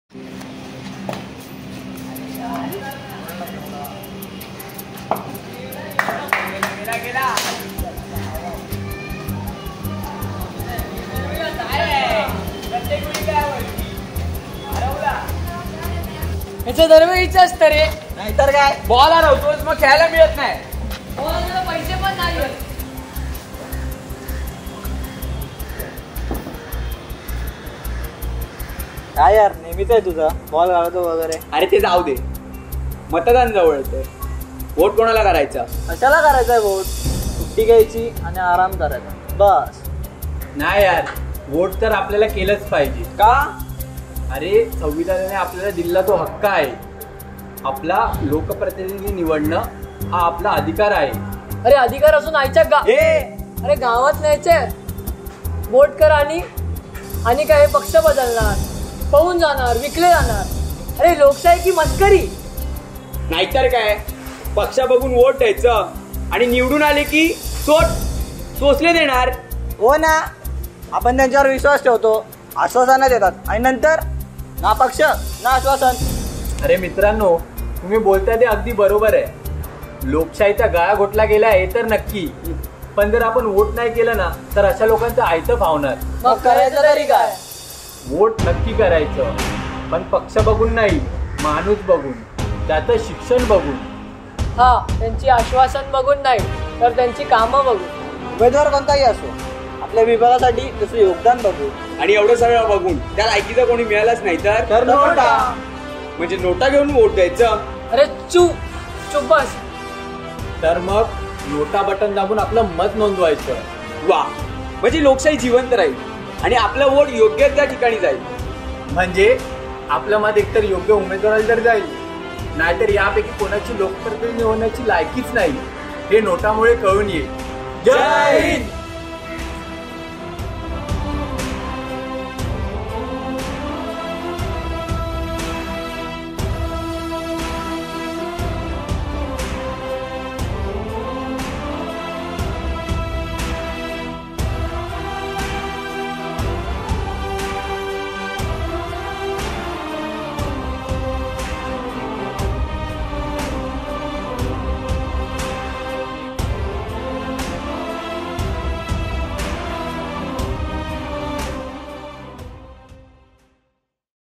JAI HIND | ELECTION 2024 | जय हिंद | मतदान २०२४ | हे दान नक्की करा !!
भारतीय राजकारणाची सद्य परिस्थिती पाहता तरुण पिढीच्या मनात निर्माण होणारे प्रश्न येथे मांडण्याचा प्रयत्न केला आहे.

Concept-Screenplay & Edit
Rohan Shivaji Tippe

cinematography
Pavan Parab & Sam Gangan

Cast
Pavan Parab
Sam Gangan
Siddhesh Patil
Yash Rahate